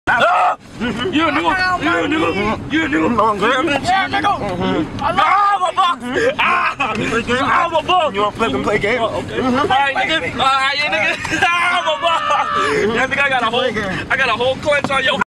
X and Ski wrestling each other
X and ski are wrestling each while driving to Denver back in 2016